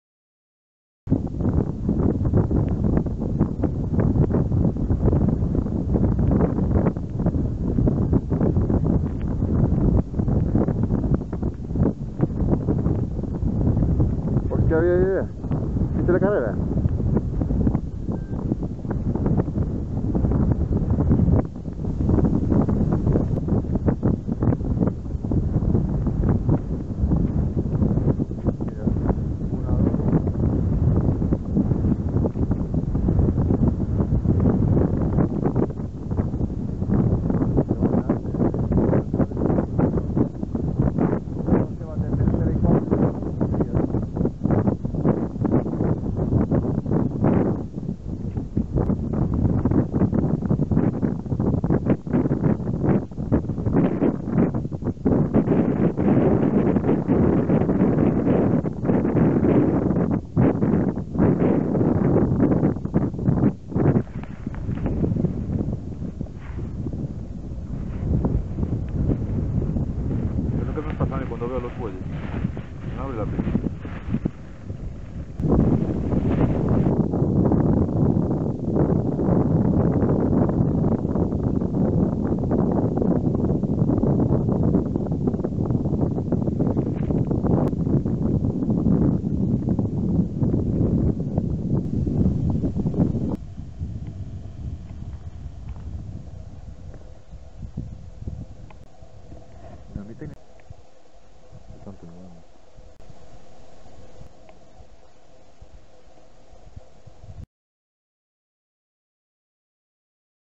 Buey almizclero, muskox (Ovibos moschatus), en Noruega
Grupo de bueyes almizcleros observado en el parque nacional de Dovrefjell, Noruega.
Expedición realizada con Óscar Villegas, Mayo 2015.